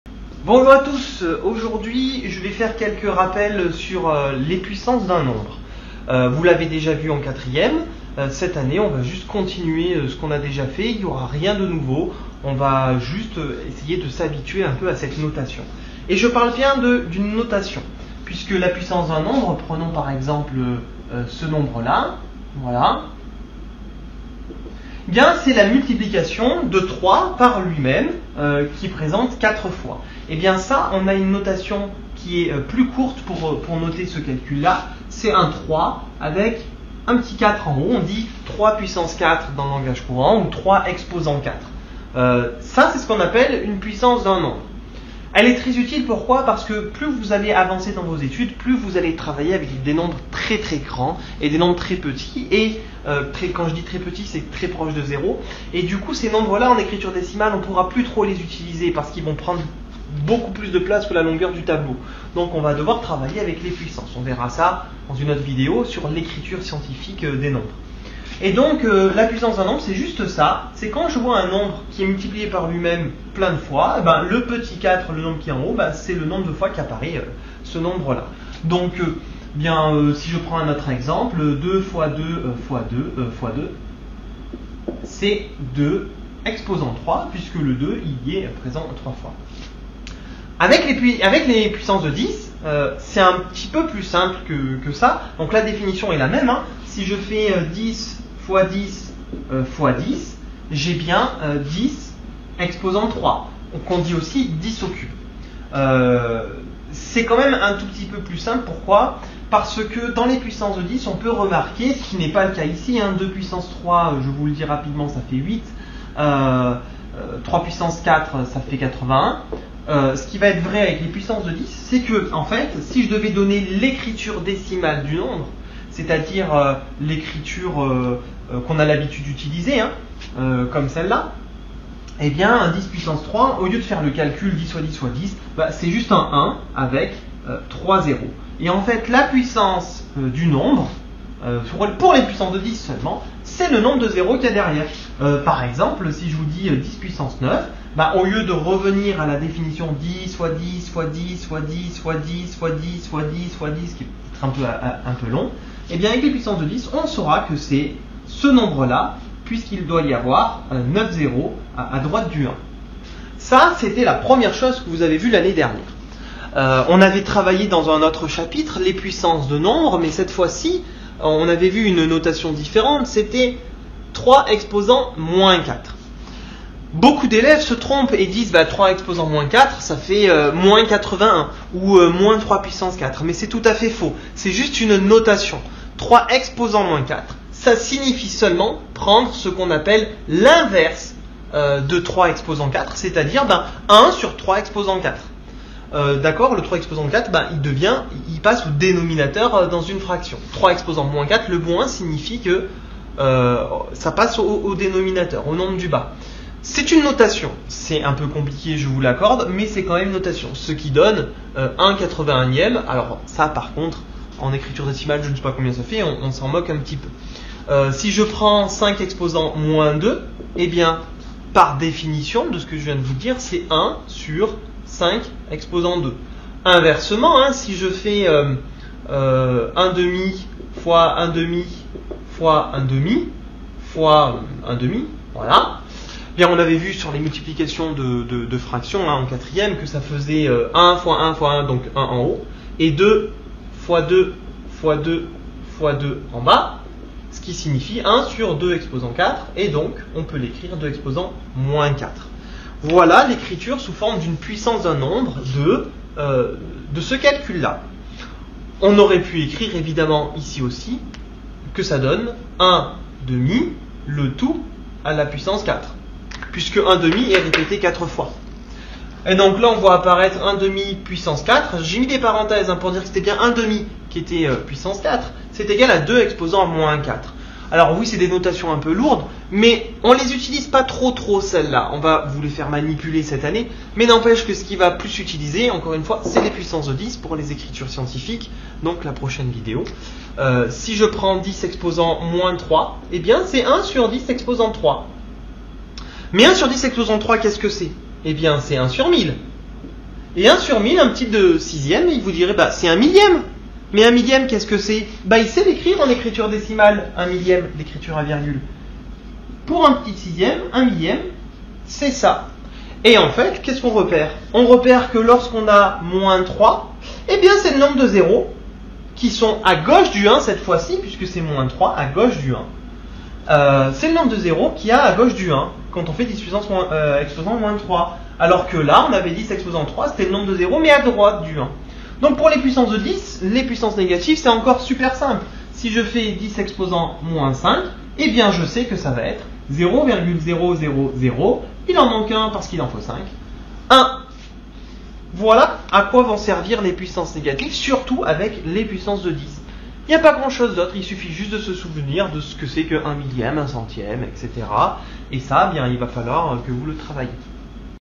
Rappel sur les puissances en 3e
On explique rapidement ce que sont les puissances d'exposant positifs et négatifs pour tous les nombre, y compris le cas particulier des puissances de 10.